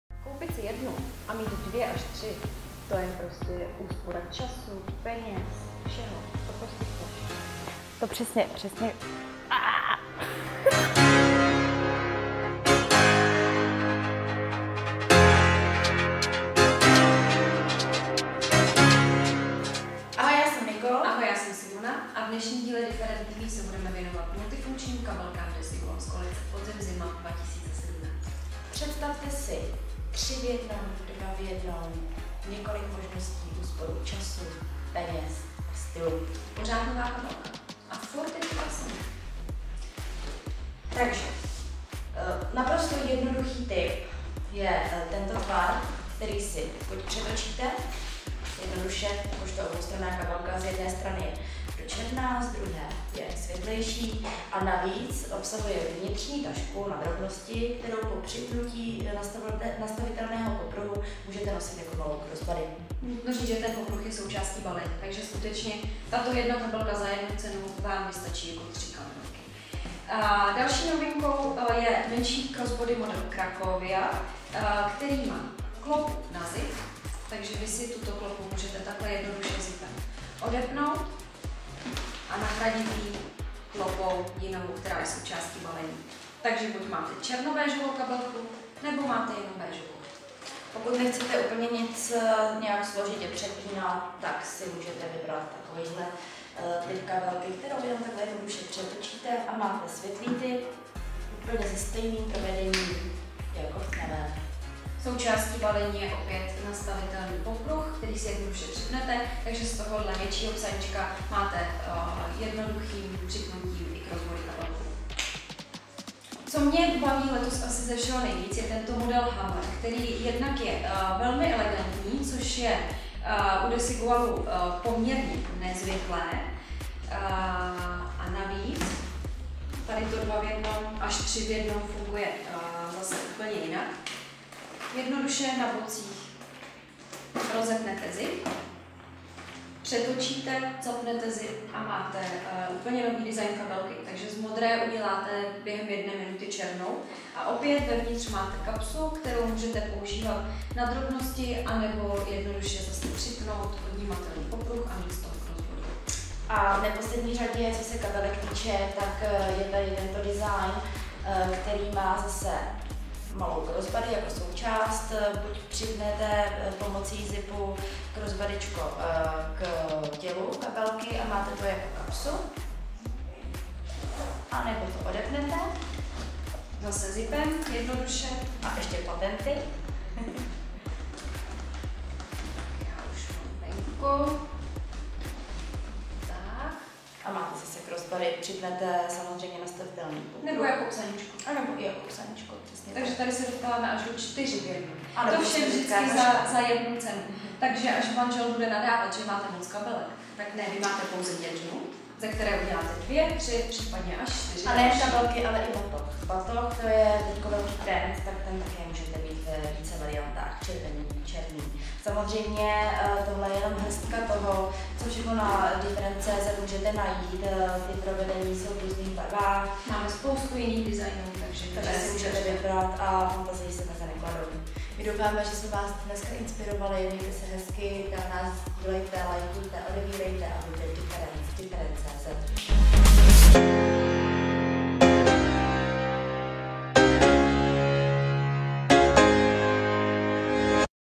MULTIFUNKČNÍ KABELKY | Different.cz
PRAKTICKÉ MULTIFUNKČNÍ KABELKY

Koupit si jednu a mít dvě až tři... No to je SEN!!!

Až vás omrzí jeden motiv, jednoduše kabelku přetočíte na ruby a rázem máte nový, neokoukaný design ;) No a až vás omrzí i ten, tak si vyndáte vnitřní tašku a tramtadadáááá - kabelka č. 3 je tu! :)

U modelu Desigual Cracovia si dokonce můžete odepnout hlavní klopu a nahradit ji jinou.

Až váš manžel bude nadávat, že máte moc kabelek... Tak néé. Vy můžete mít jen jednu. Ale vlastně tři :)